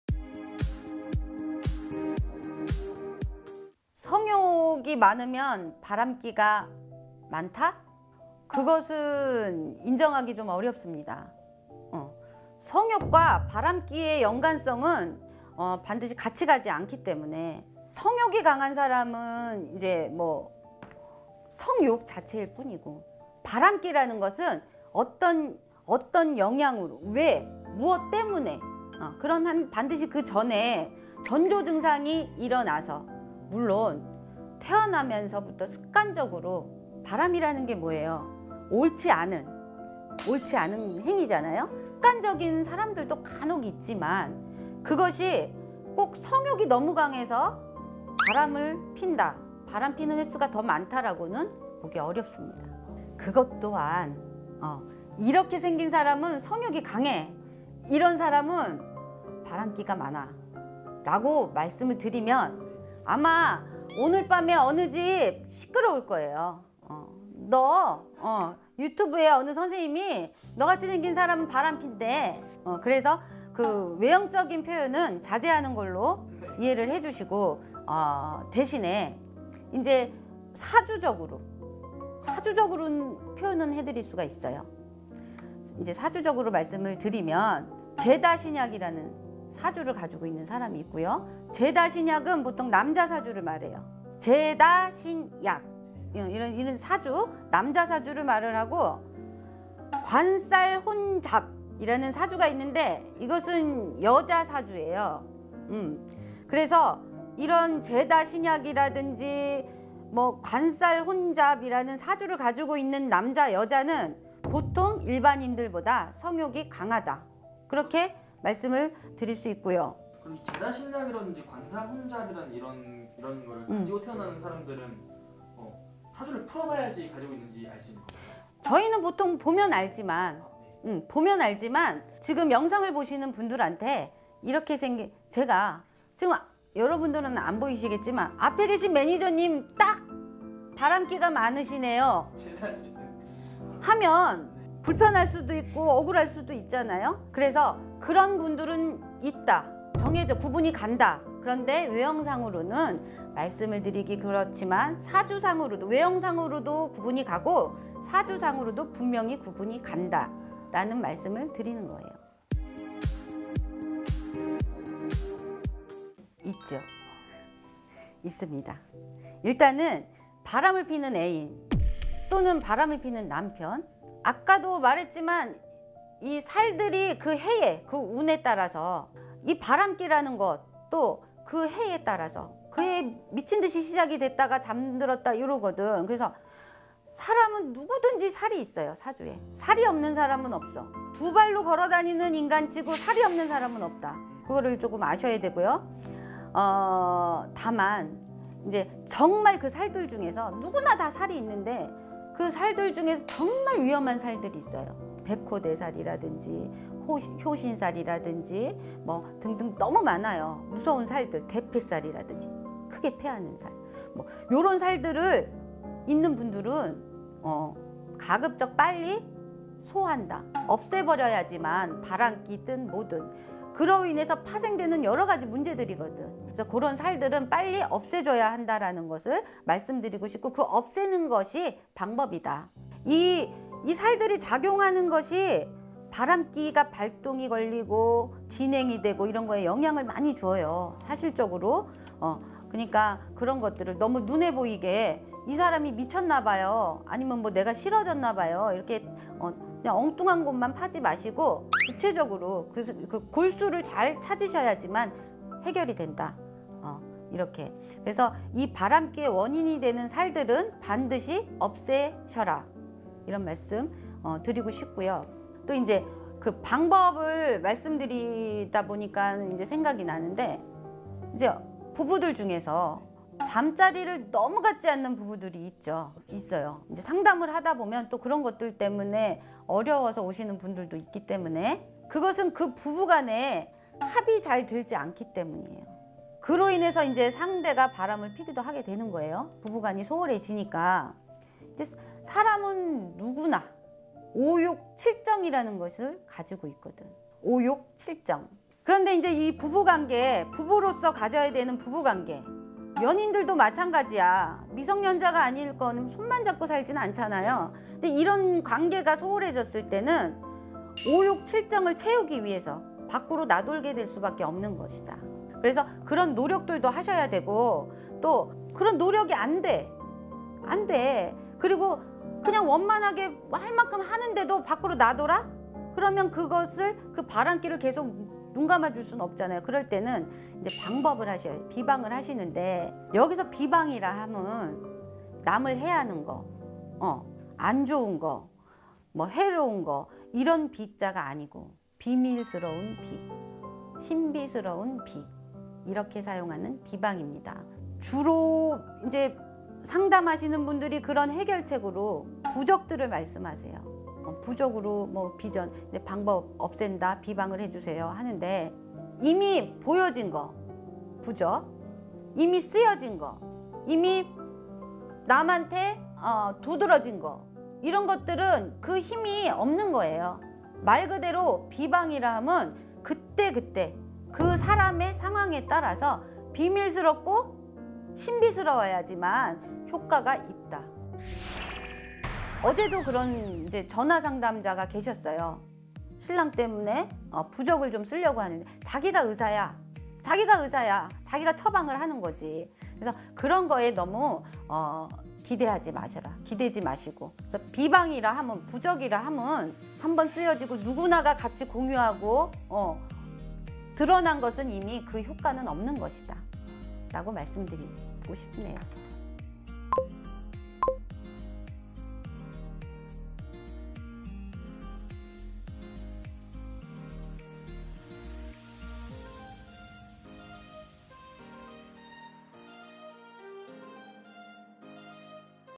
★성욕과 바람기 많은 사람의 공통적인 특징★ ''성욕이 많으면 바람끼도 많다?!'' 서울점집 삼청궁
자막

★대표블로그★
-올마이티TV